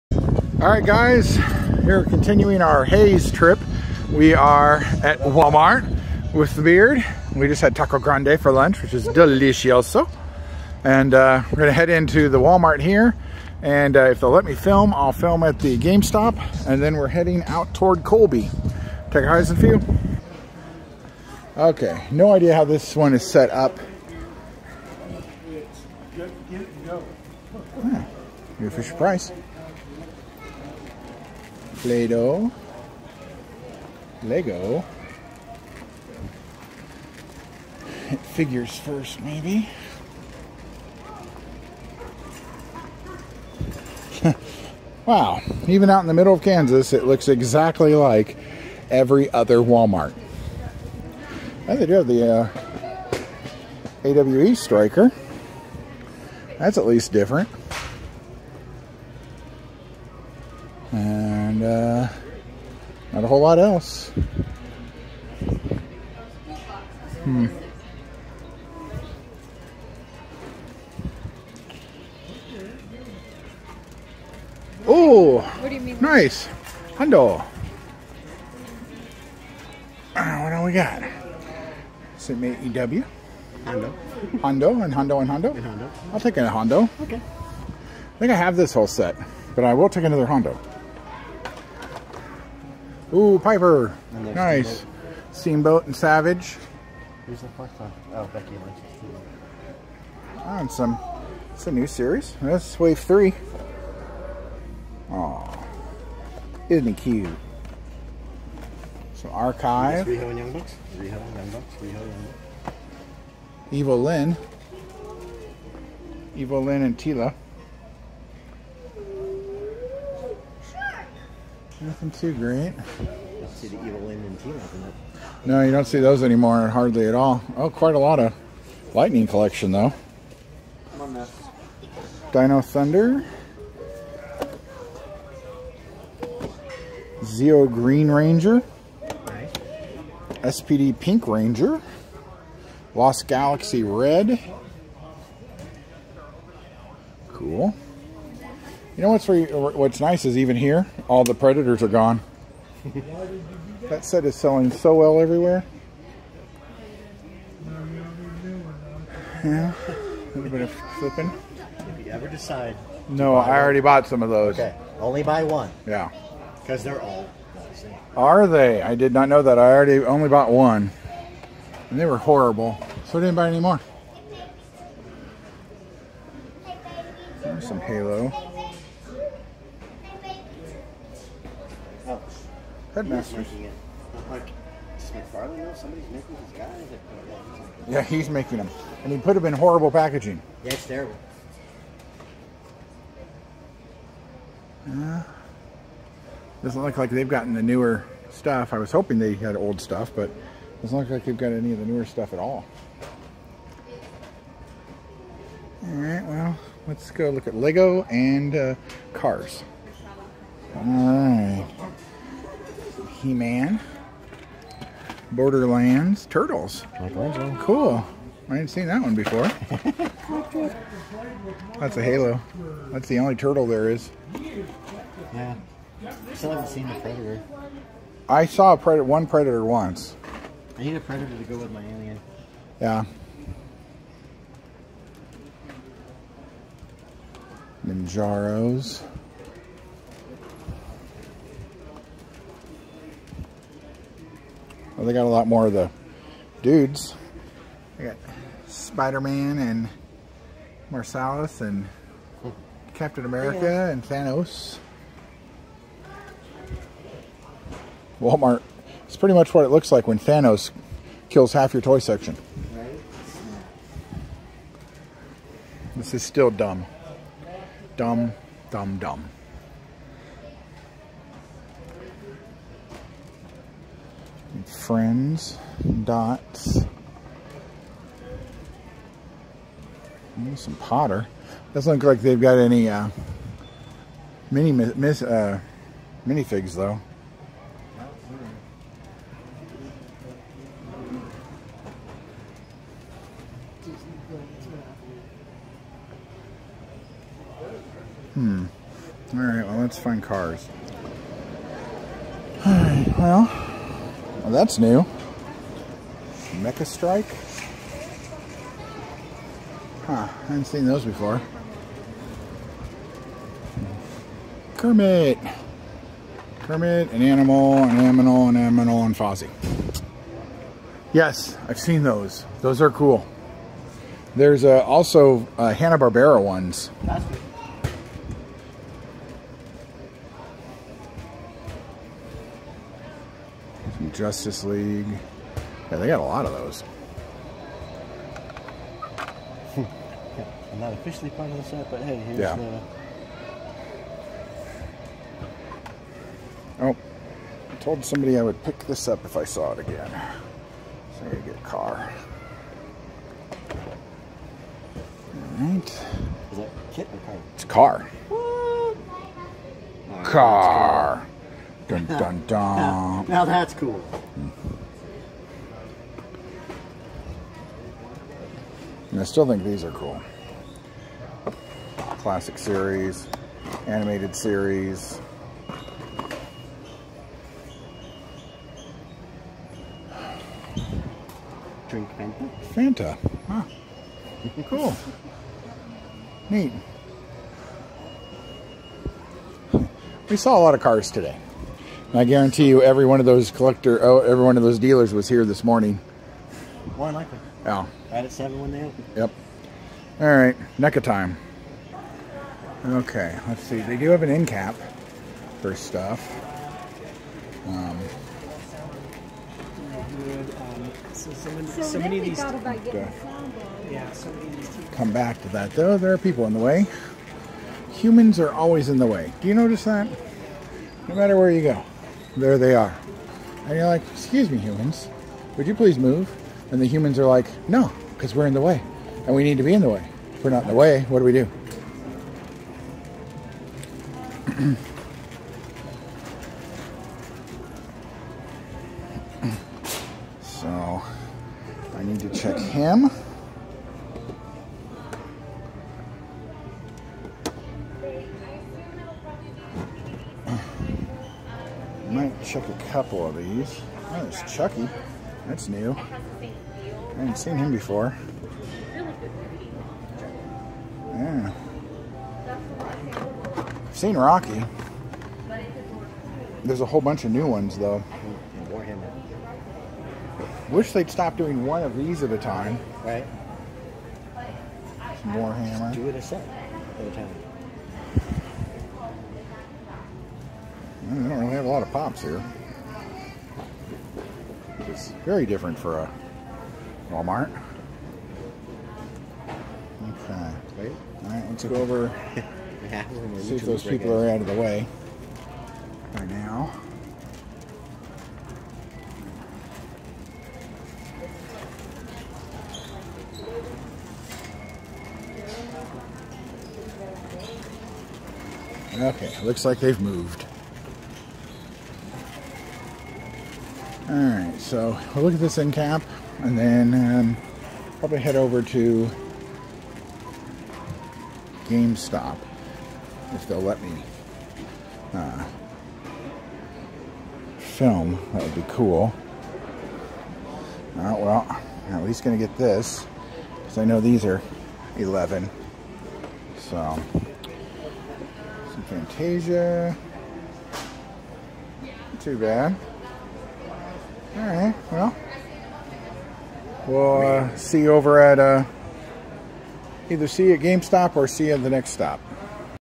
Road Trip!! Hays KS Walmart Toy Hunt
After the toy show we decided to hit a few of the Walmart's on the way back to Denver. First up is the Walmart in Hays.

• XBOX ➭ Exodus116
• Playstation➭ Exodus116